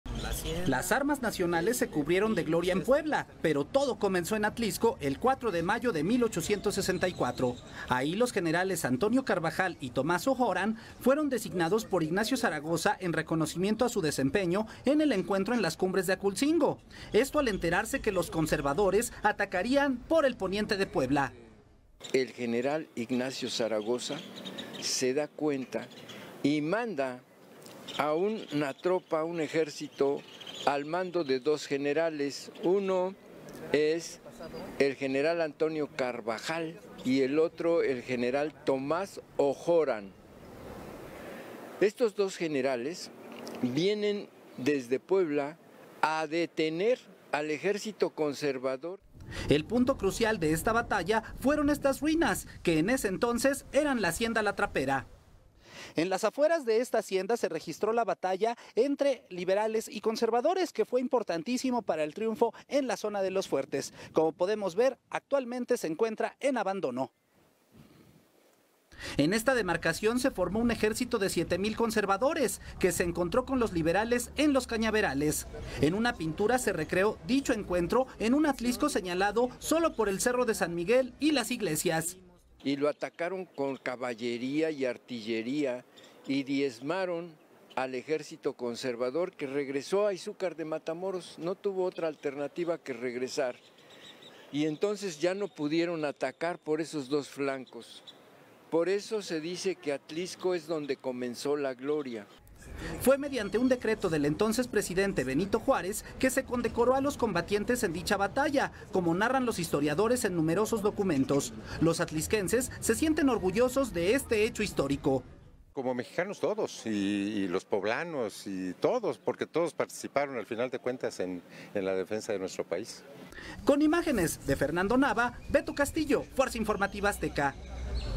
Batalla del 4 de Mayo en Atlixco
La gloria de la Batalla del 5 de Mayo comenzó en Atlixco un 4 de mayo de 1864, un dato importante para conocer más sobre la historia de Puebla.

Con información de Beto Castillo

Nuestra Programación:

ADN 40 1.2 📺
📰 ADN 40 en Puebla 20:30 - 21:30 Lunes a Viernes
⚽ADN Deportes en Puebla 21:30 - 22:00 Lunes y Miércoles